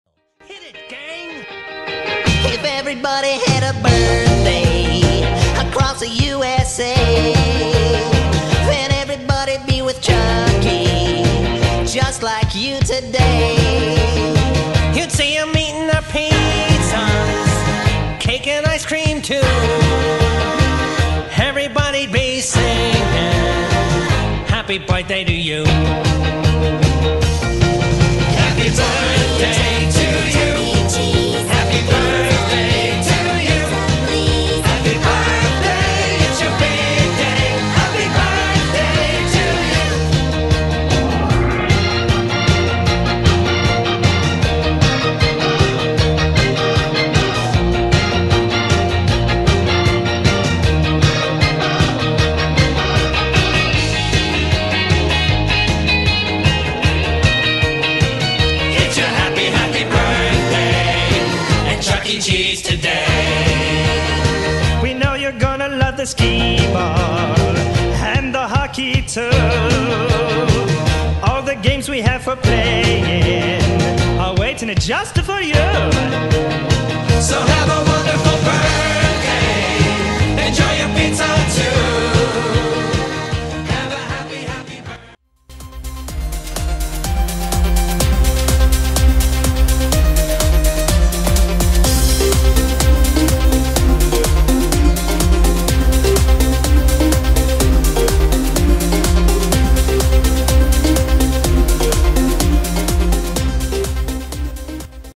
*UNBOXING* Chuck E. Cheese PTT Plush Showbiz Pizza Time Inc (1992)
This will be my last unboxing video for awhile. For this video, I purchased the Pizza Time Theatre style plush of Chuck E. More research, this version was from 1992 during the Showbiz Pizza Time Inc. run. He stands 9 inches and has glued on eyebrows and buttons. Source Hope you guys enjoy!